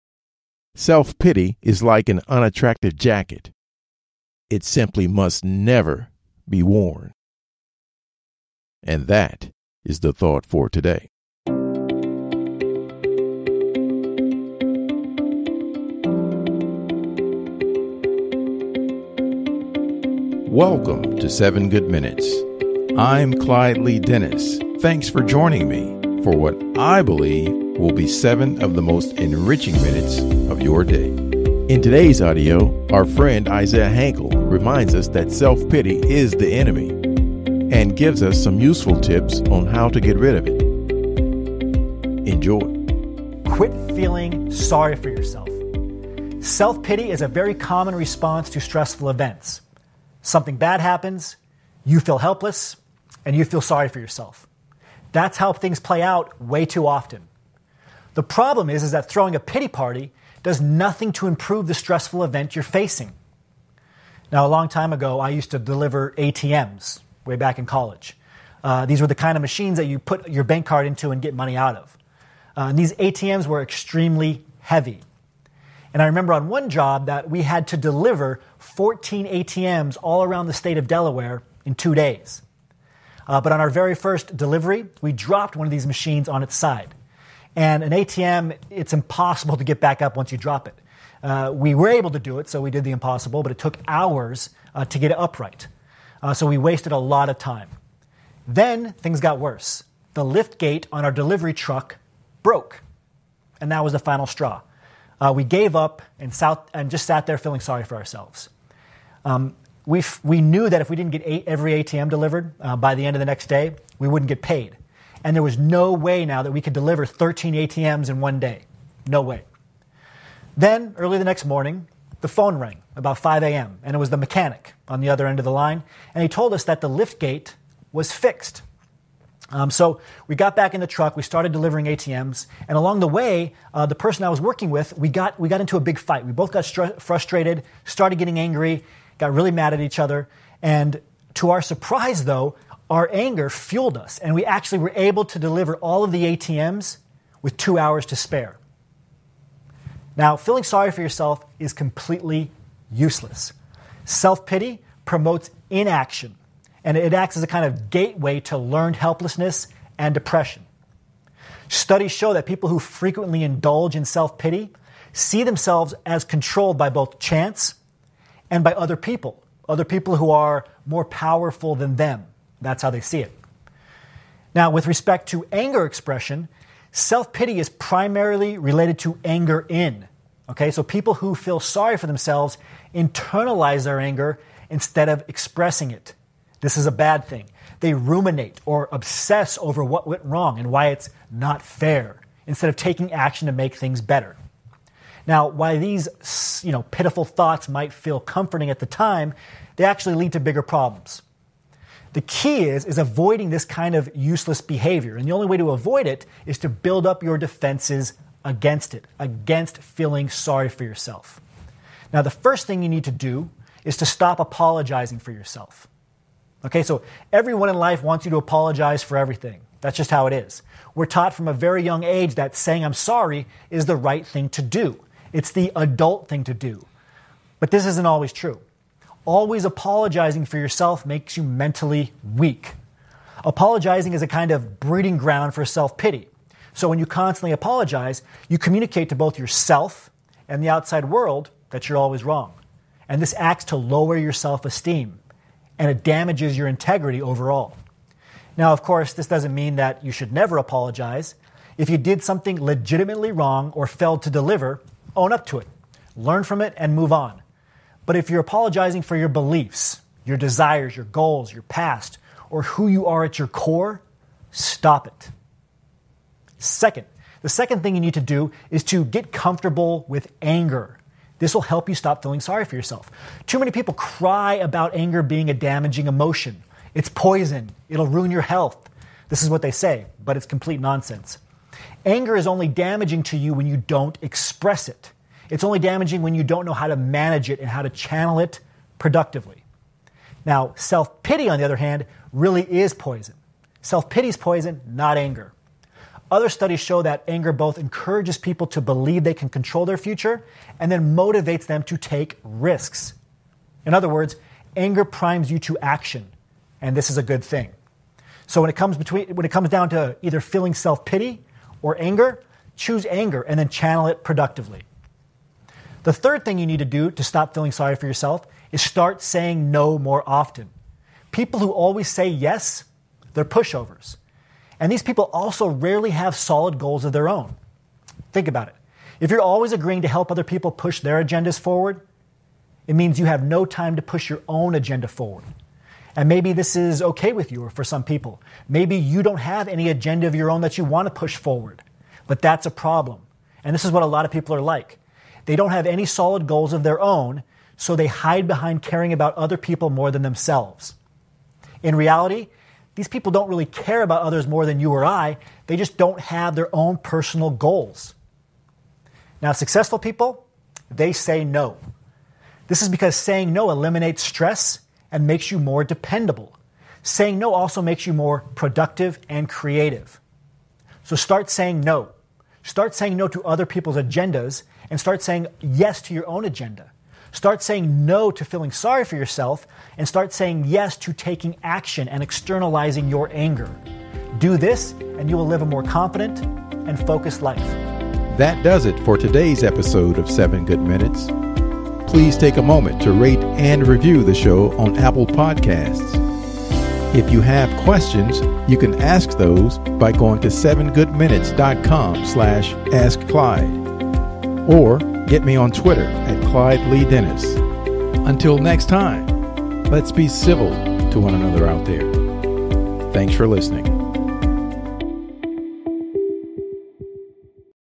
Why Self Pity Is Harming You and How To Let It Go
-=-=-=-=- In this episode of the 7 Good Minutes Podcast, our Friend Isaiah Hankel reminds us that self pity is the enemy, and gives us some tips on getting rid of it forever.

Letting Go of Self Pity

► Why Being A People Pleaser Is A Waste Of Time

► Even More About Letting Go of Self Pity